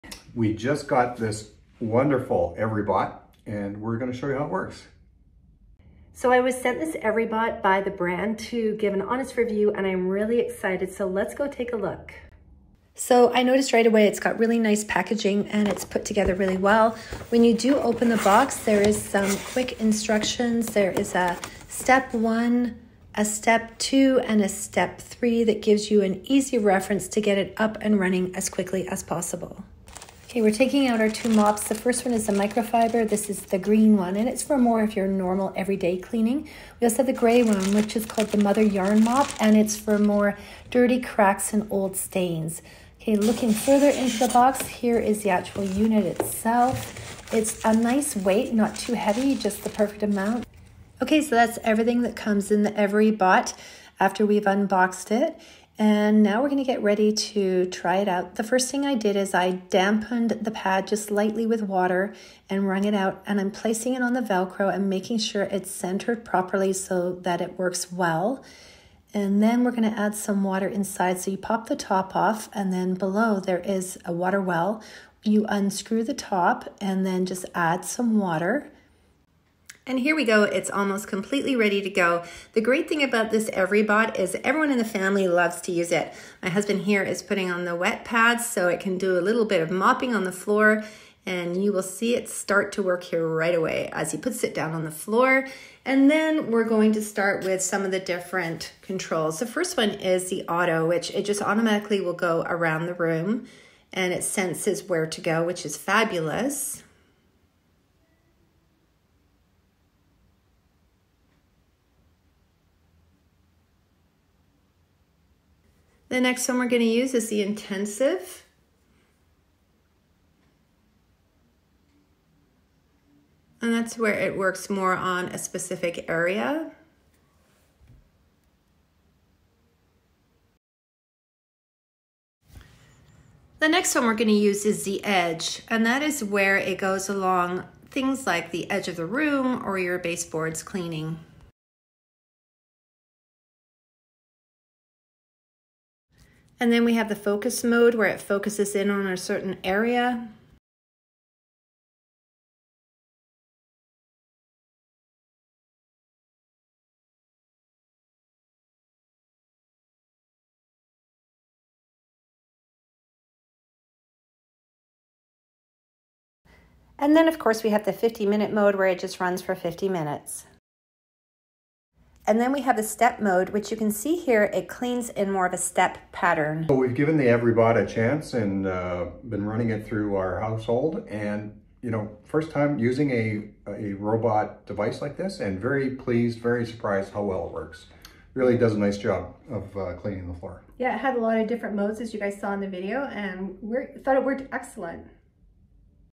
Revolutionizing Clean Floors: Introducing the EVERYBOT Edge2 Robot Mop
🌟 Embark on a Cleaning Revolution: Unveiling the EVERYBOT Edge2 Robot Mop!

Explore the next frontier in smart cleaning through the EVERYBOT Edge2 Robot Mop unboxing experience! This marvel of whisper-quiet technology promises to transform your hard floor and tile cleaning routines. Featuring a dynamic dual spin wet mop mechanism, it outpaces traditional robots, delivering unmatched efficiency and impeccable results.

🔹 Key Unboxing Features:
✨ Whisper Quiet Operation for Subtle Cleaning
✨ Dynamic Dual Spin Wet Mop Accelerates Cleaning
✨ Smart Cleaning Innovation Ensures Effectiveness
✨ Tailored for Hard Floors and Tiles
✨ State-of-the-Art EVERYBOT Technology

👉 Immerse yourself in the realm of effortless cleaning as we unveil the EVERYBOT Edge2 Robot Mop. From its discreet, whisper-quiet operation to the cutting-edge dual spin wet mop capability, this intelligent cleaner is poised to revolutionize your cleaning regimen. Embrace innovation and witness your floors undergo a remarkable transformation with each pass.